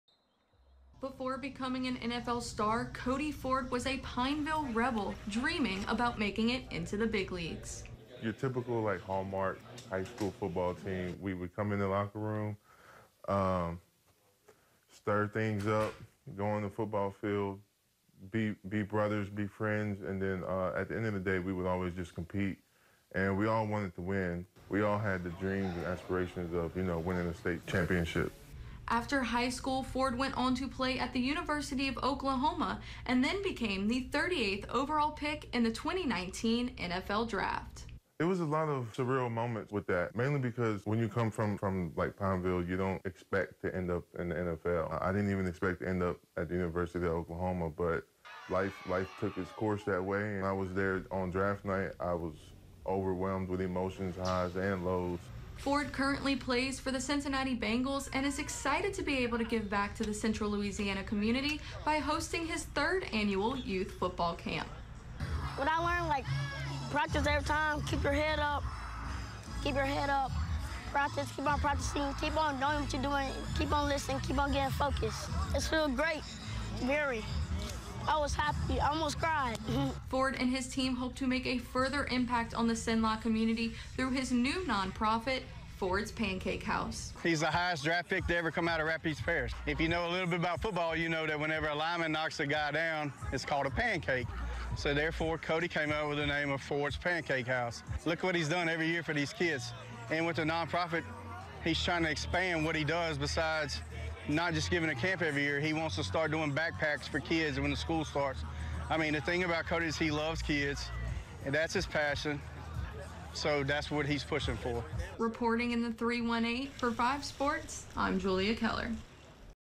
Cody Ford Youth Camp
The former Pineville Rebel is giving back to the Central Louisiana community.